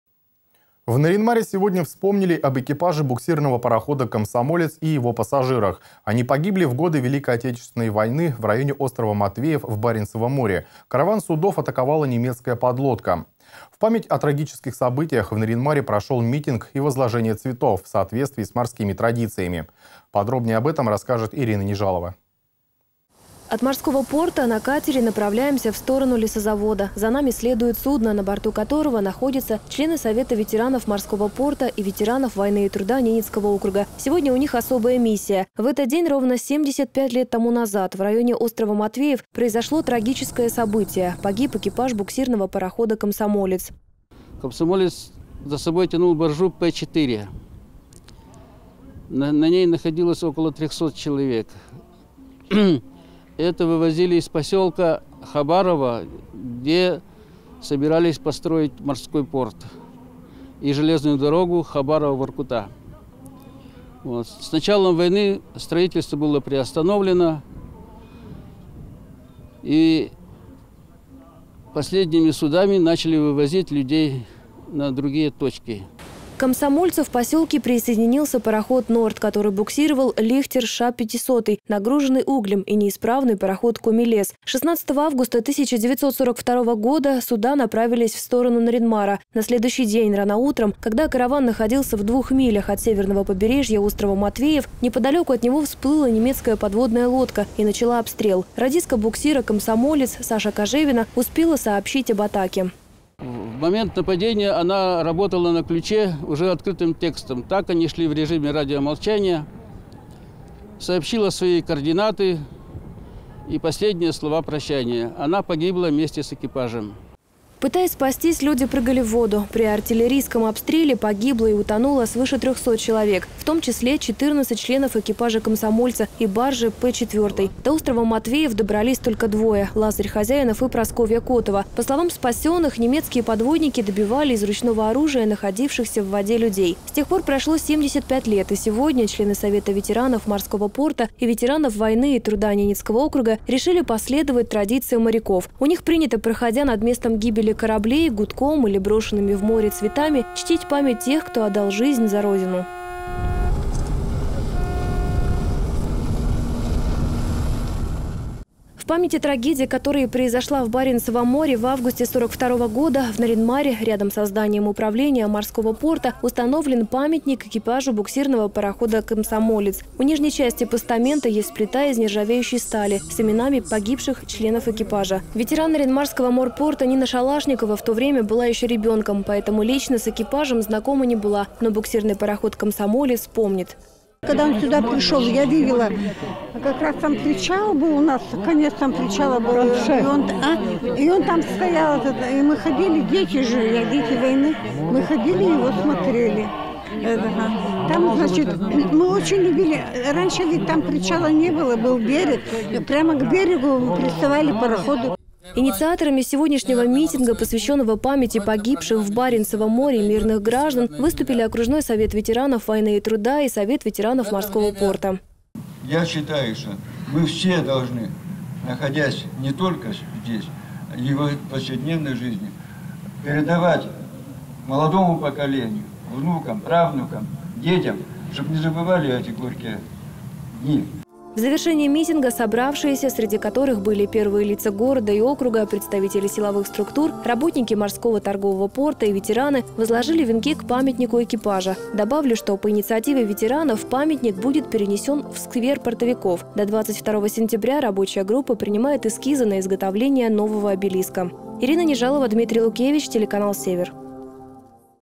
В Нарьян-Маре вспоминают об экипаже буксирного парохода «Комсомолец» и его пассажирах
В Нарьян-Маре сегодня вспоминали об экипаже буксирного парохода «Комсомолец» и его пассажирах. Они погибли в годы войны в районе острова Матвеев в Баренцевом море. Караван судов тогда атаковала немецкая подлодка. В память о трагическом событии в Нарьян-Маре прошел митинг и возложение цветов в соответствии с морскими традициями.